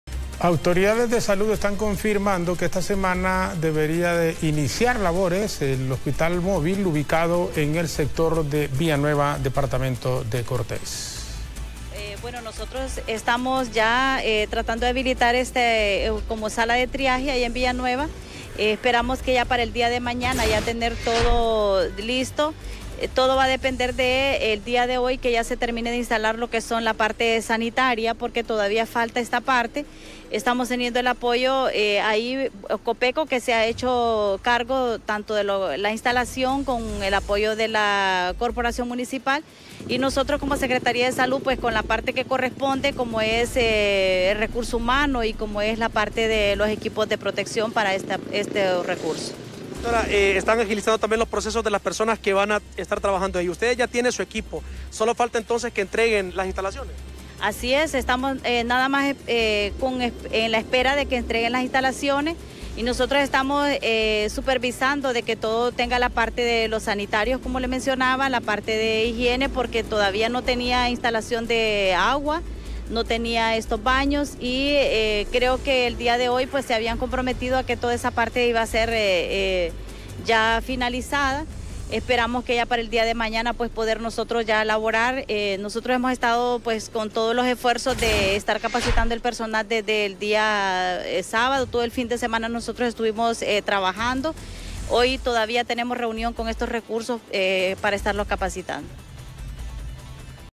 Hospital Móvil en Cortés iniciará operaciones la próxima semana
Solo se está a la espera de la instalación de baños para que esté operativo
Autoridades de salud confirman que esta semana deberían de iniciar labores en el hospital móvil, ubicado en el sector de Villanueva, Cortés.